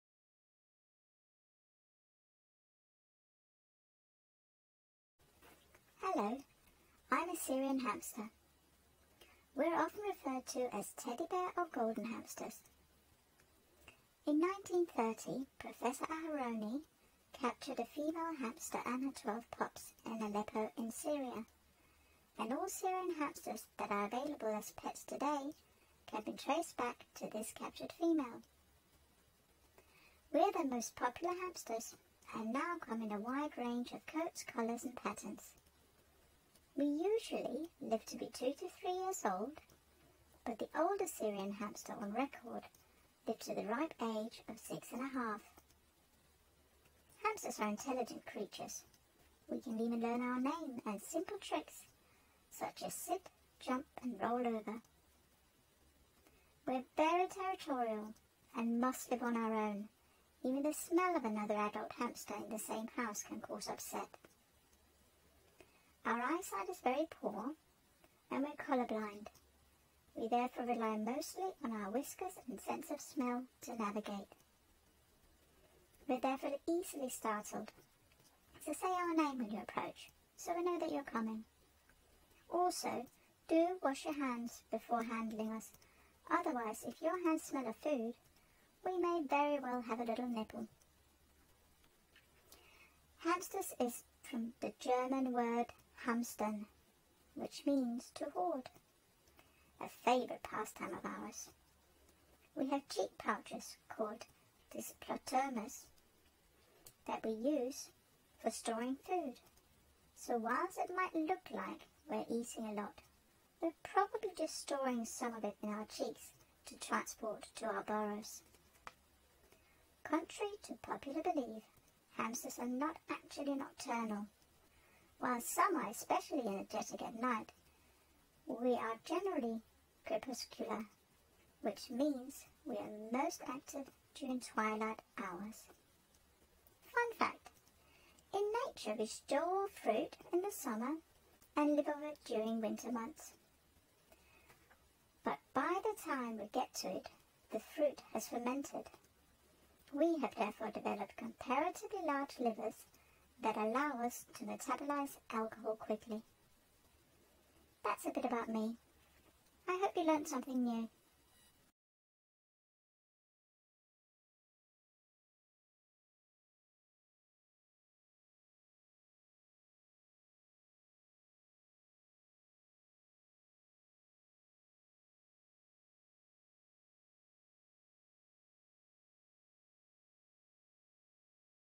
PET Talks A Furry World Syrian Hamster
A sweet Syrian Hamster giving us interesting facts.